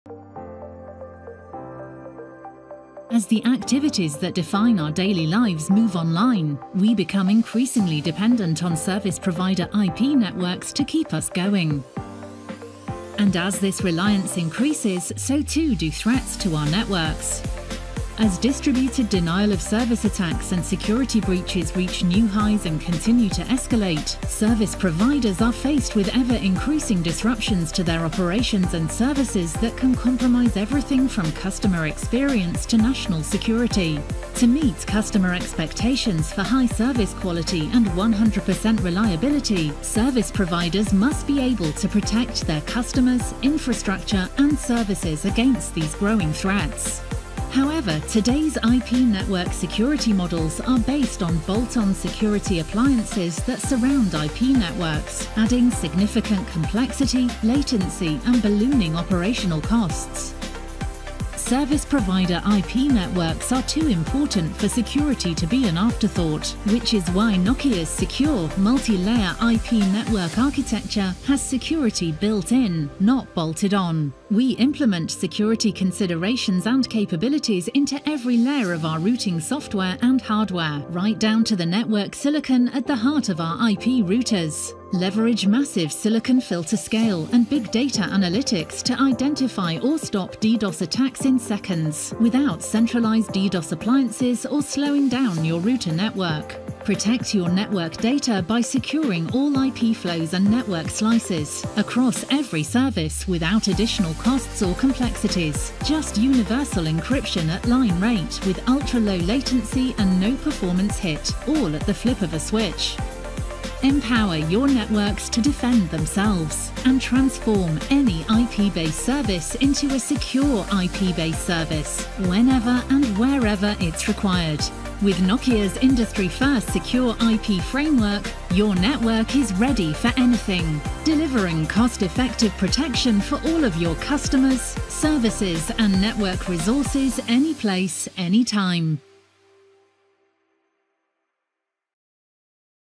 Nokia's unique, router-based approach to IP network security
Nokia’s 7750 Service Routers deliver line-rate IP network encryption and built-in DDoS protection for communication service providers and their customers.

Check out Nokia’s IP network security website to find out more